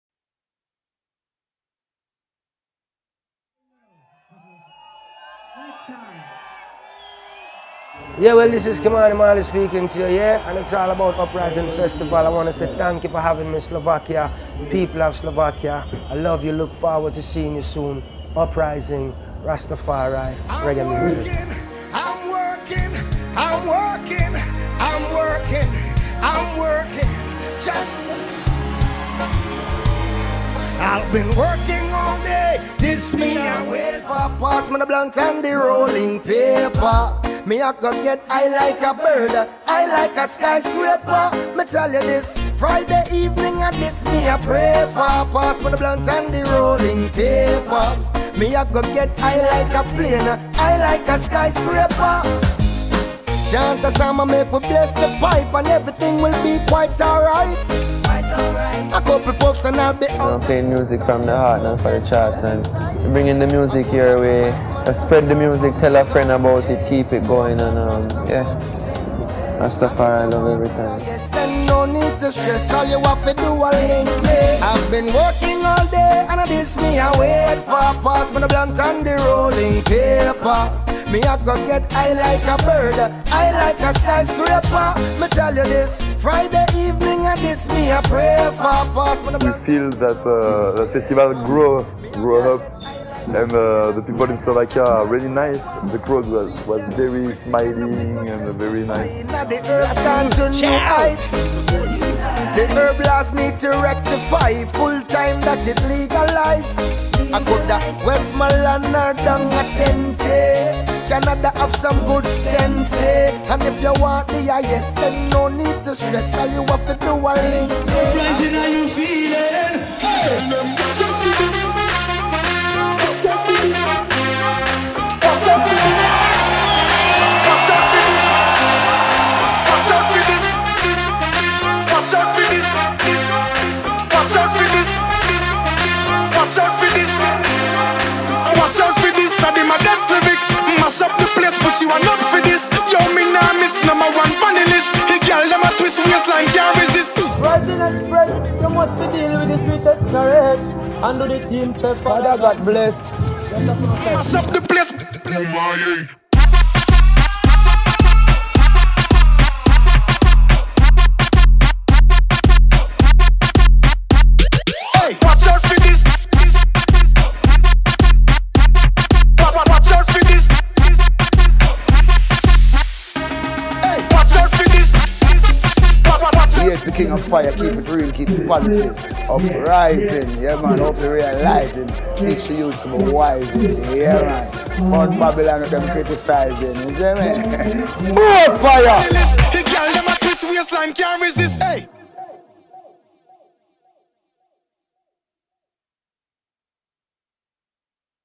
UPRISING REGGAE FESTIVAL 2013 Official Aftermovie
Uprising Reggae Festival 2013
23.,24. August 2013 @ Zlate Piesky, Bratislava, Slovakia

kamera, režia : Branislav Vincze
kamera, strih : Z matek

Uprising Reggae festival is the biggest central European festival of reggae, dancehall, hiphop, dub, dubstep, drum&bass, elektro-swing, ska, balkan a urban & world music.

In 2013 hosted names like:
KY-MANI MARLEY
BUSY SIGNAL
CAPLETON
ISRAEL VIBRATION
THE PHARCYDE
BURAKA SOM SISTEMA
PROTOJE & THE INDIGGNATION
MACKA B & MAD PROFESSOR
RANDY VALENTINE
KABAKA PYRAMID & IBA MAHR
BENNY PAGE
MILLION STYLEZ